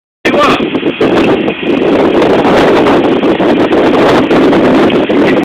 Joey! - Can Open windows!
Watch as joey closes his car window